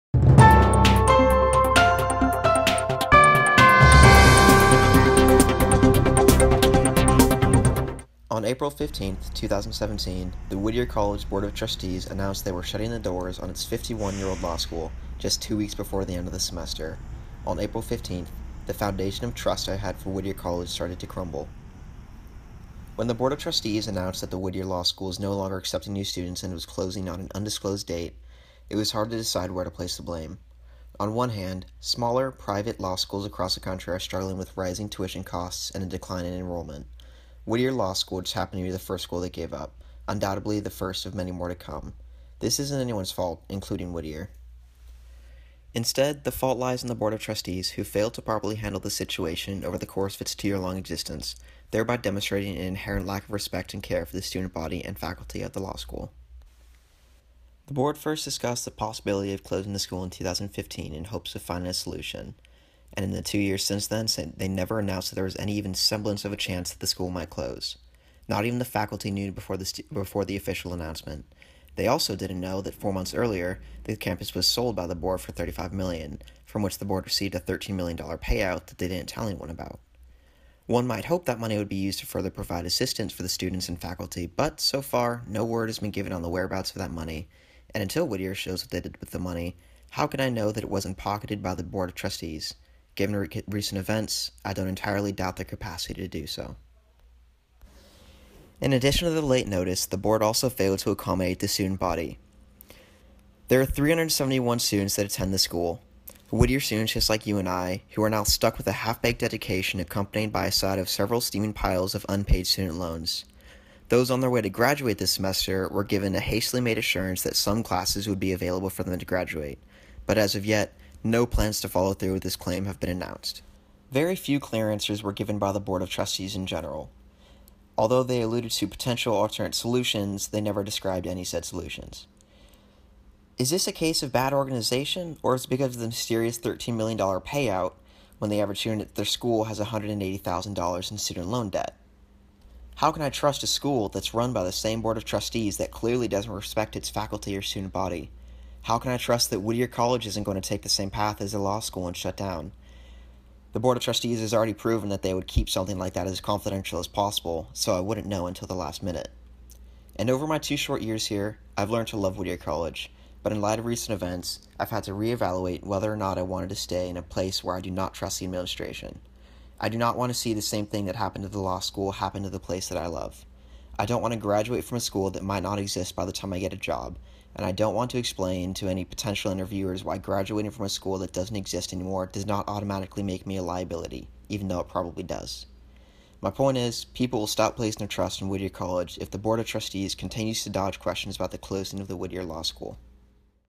Sociology Final
Sources

CBS Los Angeles  "Whittier Law School to Close Down"
Inside Highered  "What Comes After Whittier Shutdown?"
New York Times  "Whittier Law School to Close"
US News  "Whittier College to Close Law School"
"Whittier Law School Collapse Won't Be the Last"